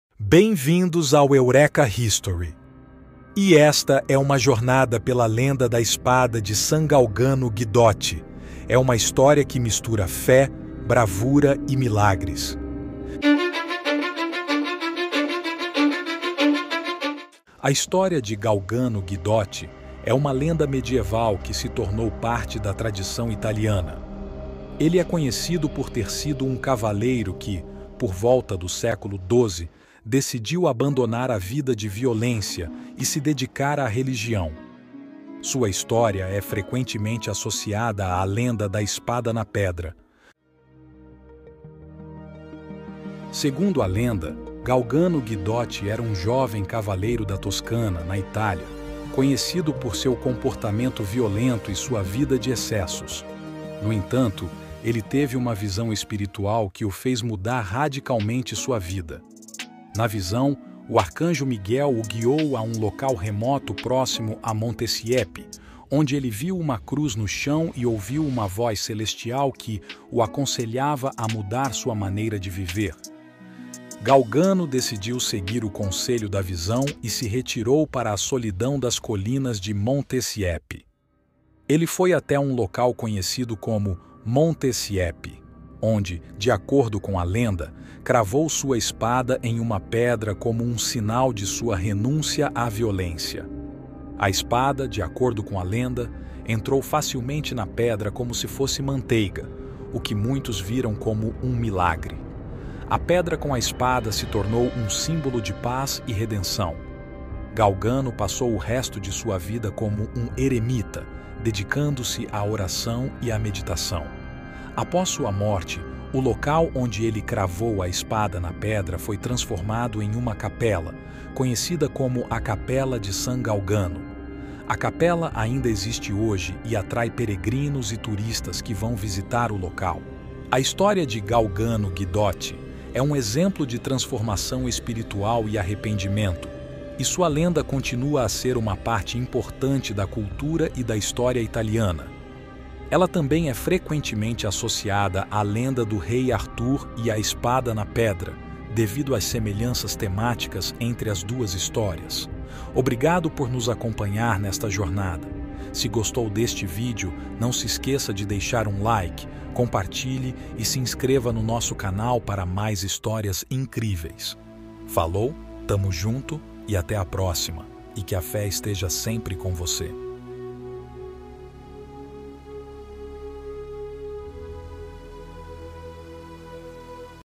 A Espada Misteriosa na Pedra de São Galgano Guidotti
A história de  São Galgano Guidotti é uma lenda medieval que se tornou parte da tradição italiana. Ele é conhecido por ter sido um cavaleiro que, por volta do século XII, decidiu abandonar a vida de violência e se dedicar à religião. Sua história é frequentemente associada à lenda da Espada na Pedra.